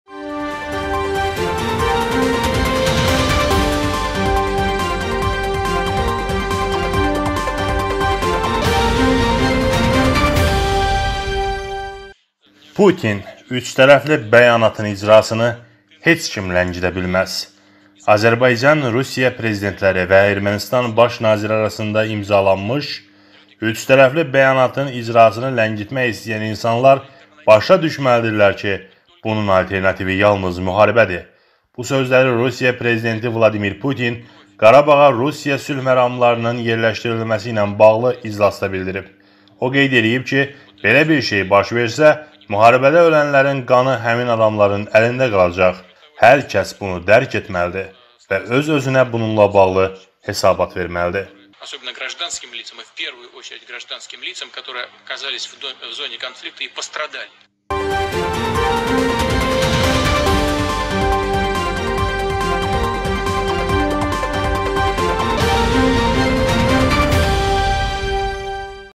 Putin od puskurdu: Beyanati hec kimi poza bilmez, yoxsa qan ve muharibe olacaq
Putin: Üçtərəfli bəyanatın icrasını heç kim ləngidə bilməz, alternativi qan və müharibədir

Azərbaycan, Rusiya prezidentləri və Ermənistan baş naziri arasında imzalanmış üçtərəfli Bəyanatın icrasını ləngitmək istəyən insanlar başa düşməlidirlər ki, bunun alternativi yalnız müharibədir.

Bu sözləri Rusiya Prezidenti Vladimir Putin Qarabağa Rusiya sülhməramlılarının yerləşdirilməsi ilə bağlı iclasda bildirib.

O qeyd edib ki, belə bir şey baş versə, müharibədə ölənlərin qanı həmin adamların əlində qalacaq:

“Hər kəs bunu dərk etməlidir və öz-özünə bununla bağlı hesabat verməlidir”.